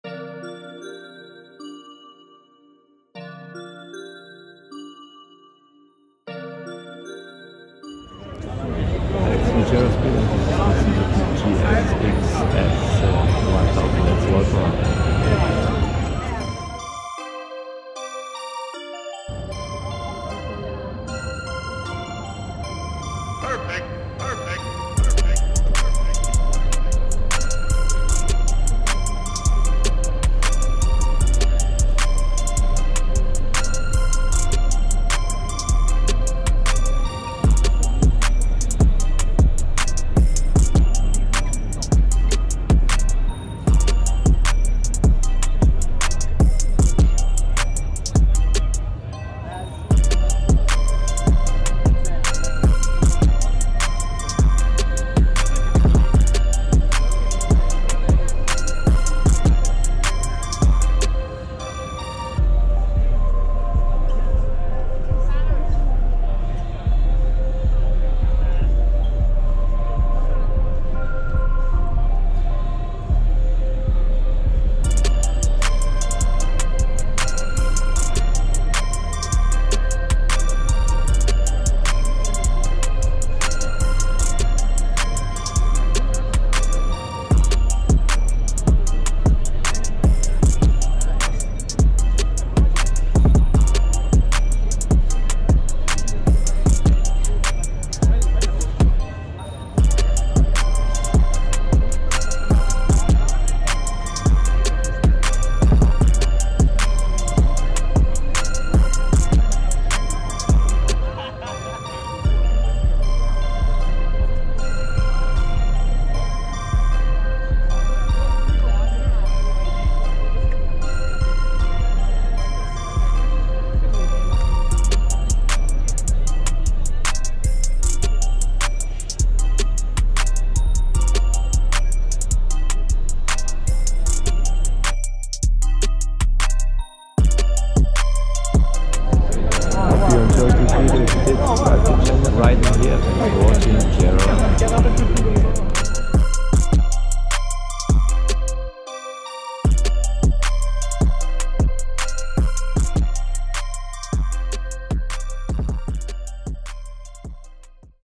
SUZUKI GSX-S 1000 F STREET SUPERBIKE WALKAROUND
jaro speed
Suzuki GSX-S 1000F
999ccm / 4cyl. / 110kw / 150hp / 108NM
seat height: 810mm
weight: 210kg
2019